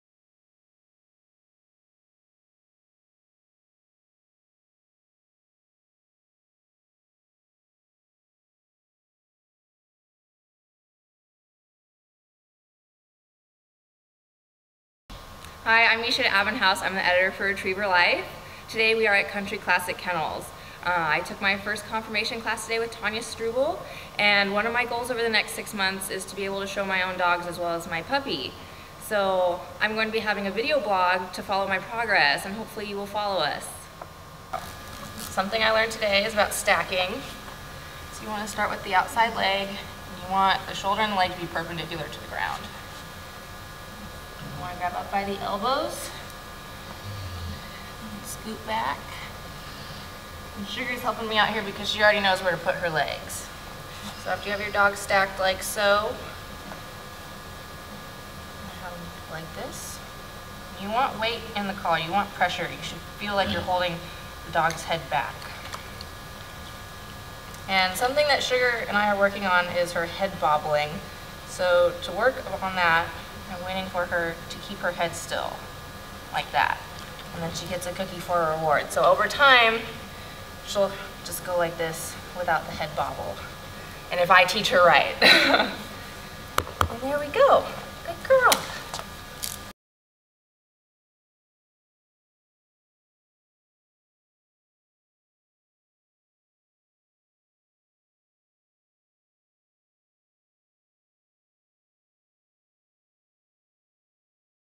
Retriever Life - Misha Abbenhouse Dog Conformation Training 1st Class
Today at Country Classic Kennels I participated in my first conformation class, taught by Tonya Struble. I brought my dog Sugar Kane to help teach me the basics. It was a lot of fun and I look forward to learning more. You can follow our adventures and progress through Retriever Life.